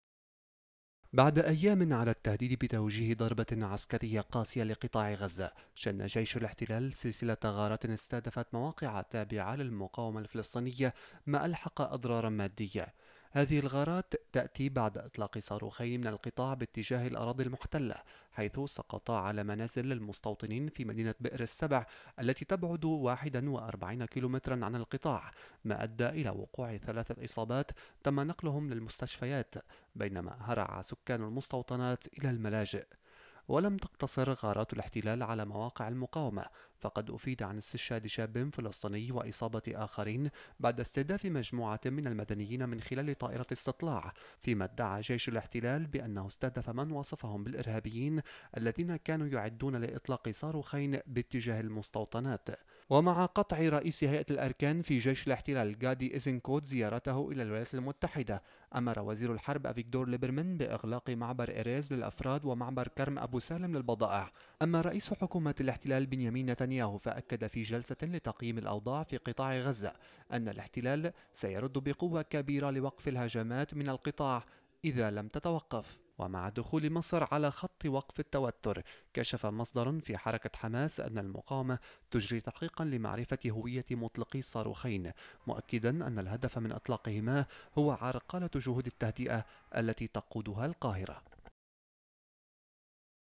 العدو الاسرائيلي يستبيح قطاع غزة
بعد أيام على التهديد بتوجيه ضربة عسكرية قاسية لقطاع غزة، شن جيش الإحتلال سلسلة غارات استهدفت مواقع تابعة للمقاومة الفلسطينية ما ألحق أضرار مادية.

هذه الغارات تأتي بعد إطلاق صاروخين من القطاع باتجاه الأراضي المحتلة حيث سقطا على منازل للمستوطنين في مدينة بئر السبع التي تبعد واحدا وأربعين كلم عن القطاع ما أدى الى وقوع ثلاثة إصابات تم نقلهم للمستشفيات بينما هرع سكان المستوطنات الى الملاجىء.

ولم تقتصر غارات الإحتلال على مواقع المقاومة فقد أفيد عن استشهاد شاب فلسطيني وإصابة آخرين بعد استهداف مجموعة من المدنيين من خلال طائرة استطلاع فيما ادعى جيش الإحتلال بأنه استهدف من وصفهم بالإرهابيين الذين كانوا يعدون لإطلاق صاروخين باتجاه المستوطنات.

ومع قطع رئيس هيئة الأركان في جيش الإحتلال غادي أيزنكوت، زيارته إلى الولايات المتحدة، أمر وزير الحرب إفيغدور ليبرمان، بإغلاق معبر إيرز للأفراد، ومعبر كرم أبو سالم للبضائع.

أما رئيس حكومة الإحتلال بنيامين نتنياهو فاكد في جلسة لتقييم الأوضاع في قطاع غزة أن الإحتلال سترد بقوة كبيرة لوقف الهجمات من قطاع غزة إذا لم تتوقف.

ومع دخول مصر على خط وقف التوتر، كشف مصدر في حركة حماس أن المقاومة تجري تحقيقا لمعرفة هوية مطلقي الصاروخين، مؤكدا أن الهدف من إطلاقهما هو عرقلة جهود التهدئة التي تقودها القاهرة.